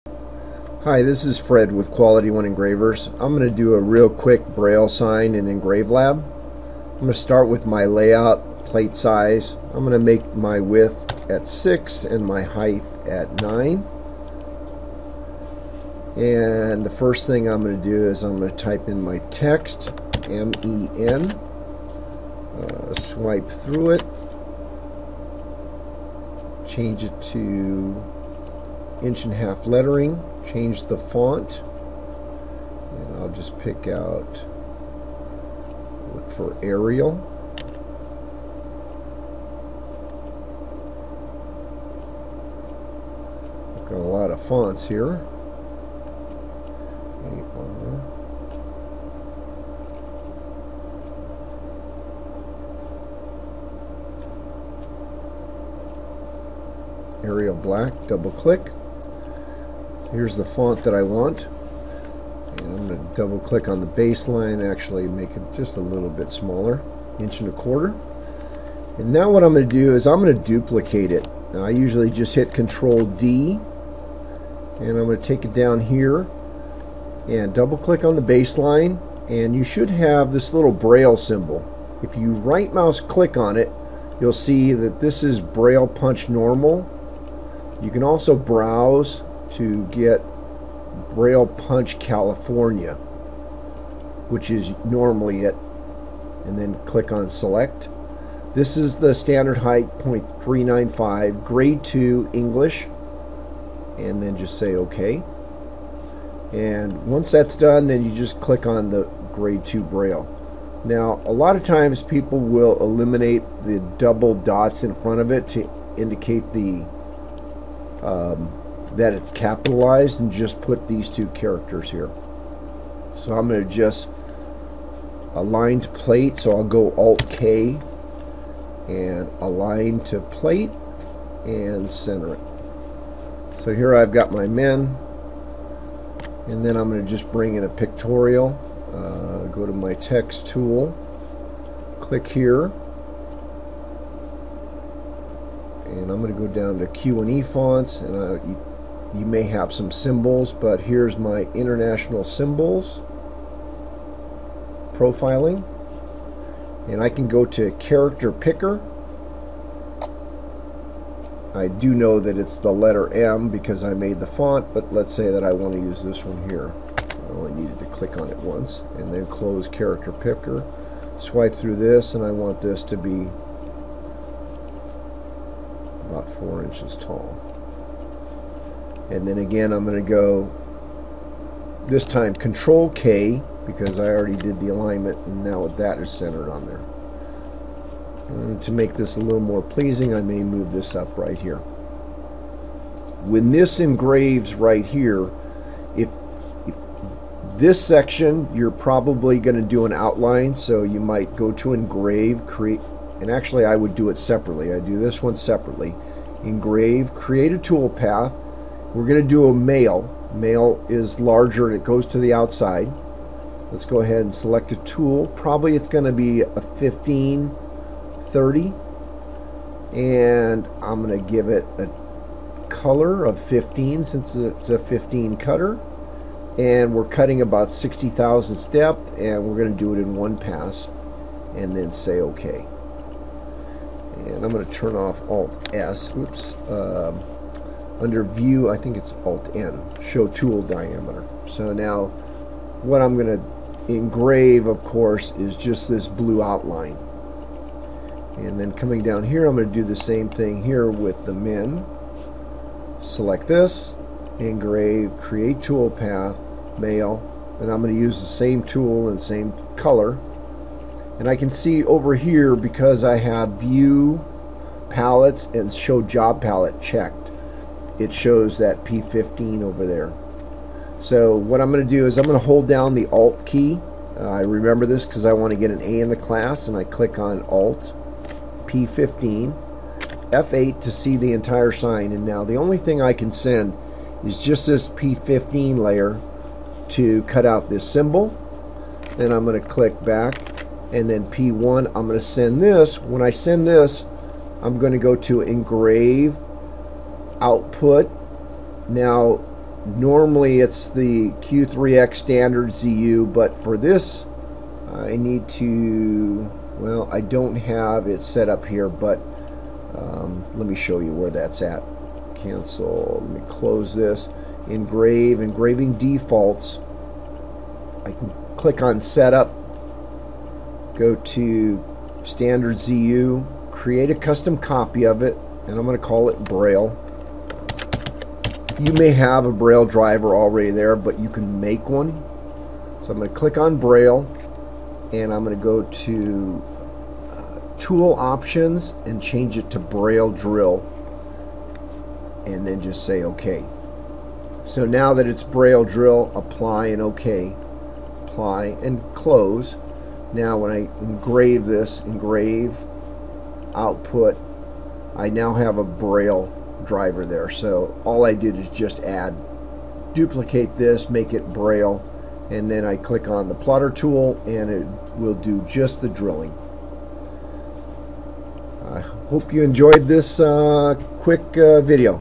Braille Sign in Engravelab.mp4
EngraveLab Training - Quality One Engravers (Q1E)  Braille Simple Sign setup